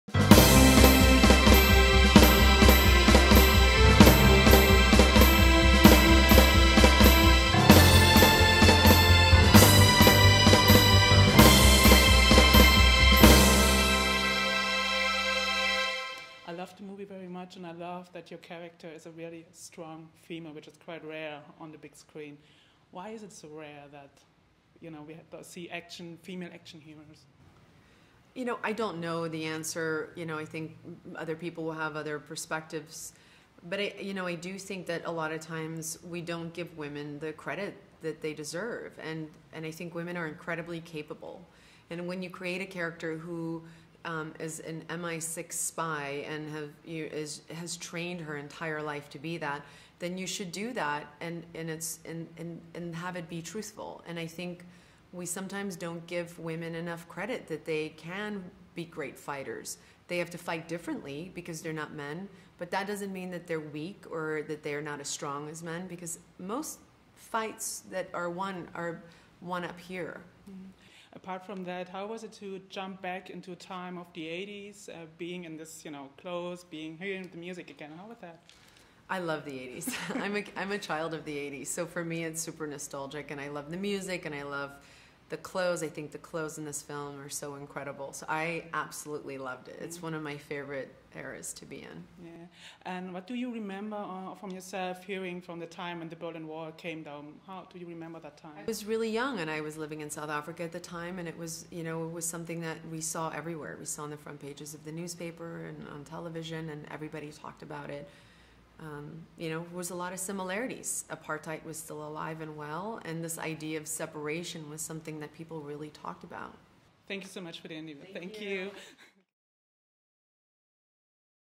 Interview Atomic Blonde Charlize Theron
Berlin, November 1989 - die Berliner Mauer wird unter dem Druck der Proteste zusammenbrechen. Aber darum geht es nicht in "Atomic Blonde". Vielmehr erzählt der Film die Geschichte der Agentin Lorraine und ihrer Suche nach Beweisen für einen Doppelagenten im Spiel der Ost-West-Mächte. Julie, the Movie Girl, spricht mit Hauptdarstellerin Charlize Theron über die Rolle, weibliche Actionhelden und die Mode der 80er Jahre.